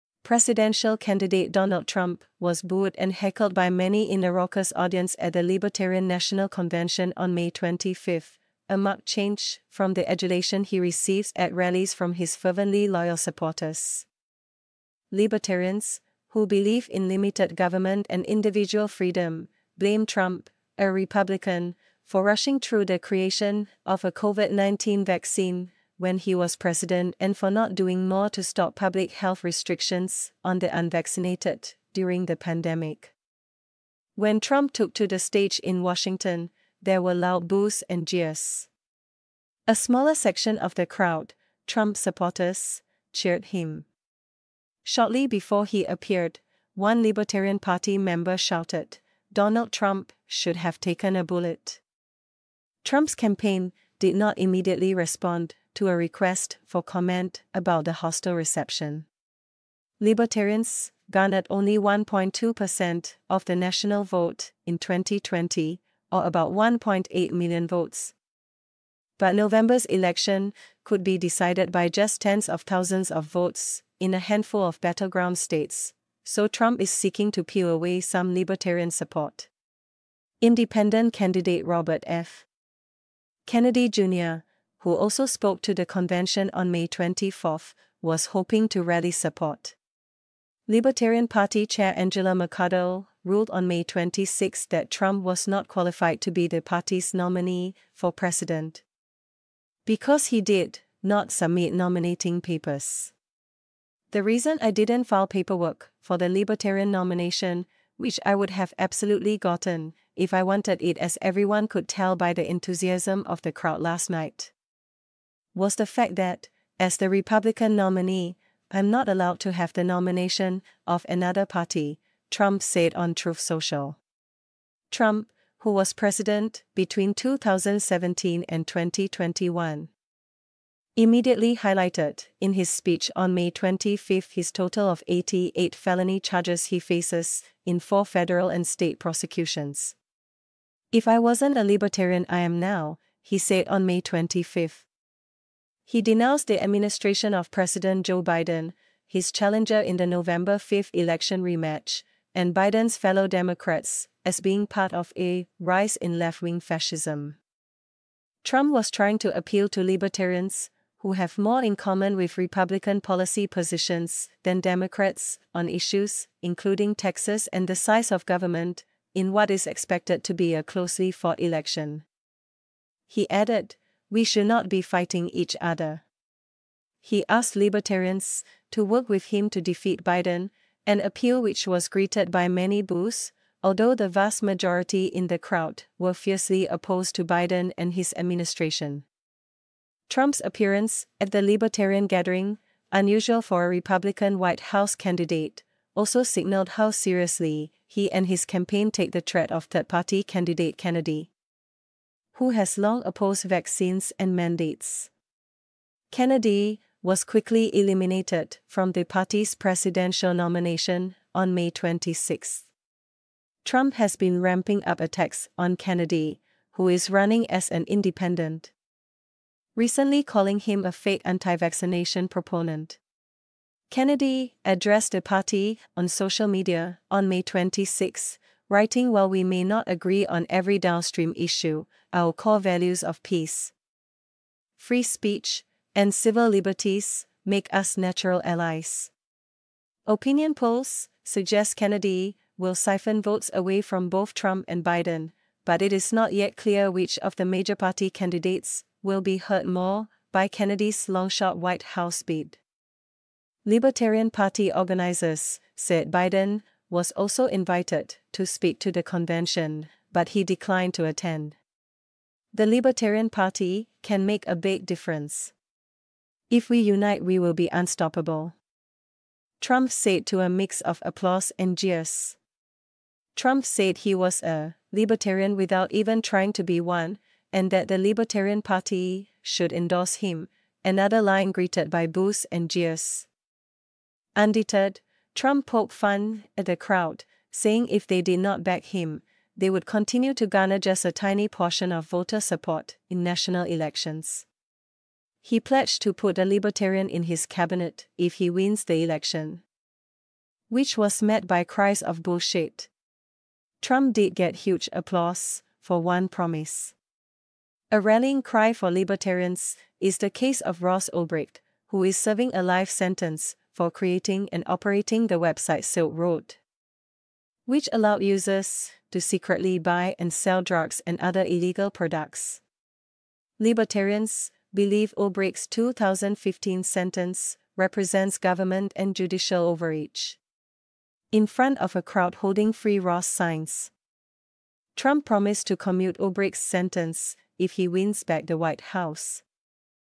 Trump booed and heckled by raucous crowd at Libertarian convention
💗📌 Singapore